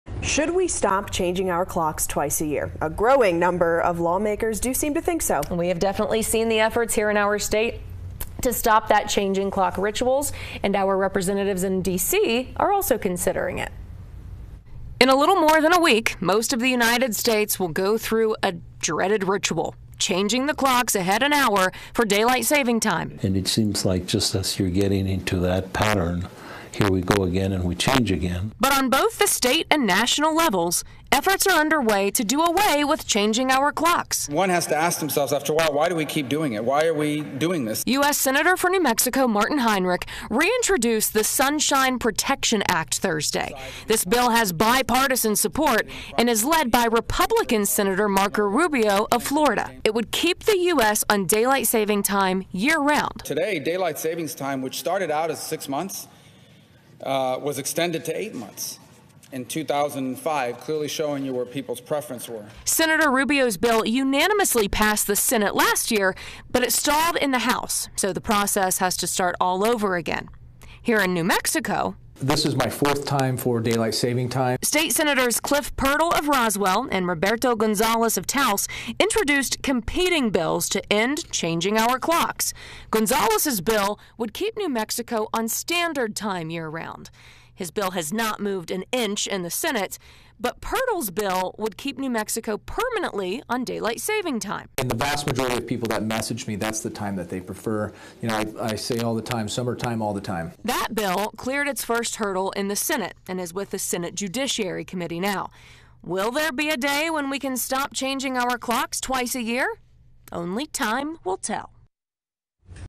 Permanent daylight saving time bill gets renewed push
On both the state level and on the national level, efforts are underway to get rid of the practice of changing the clocks